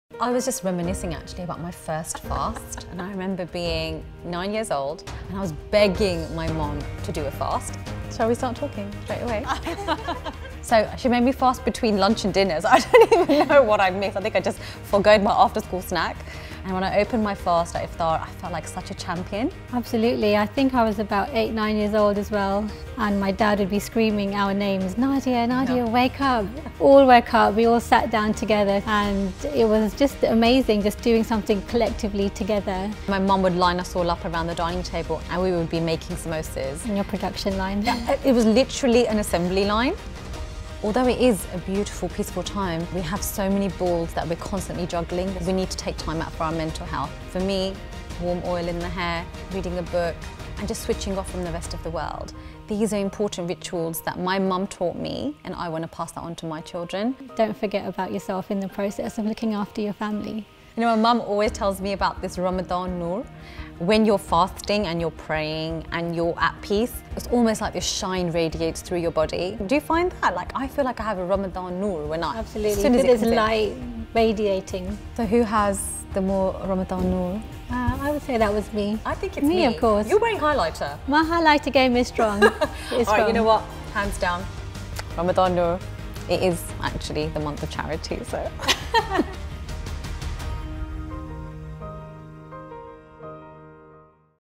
Vatika Ramadan 2021
The campaign is about three distinct personalities, sharing stories about their FIRST FAST during Ramadan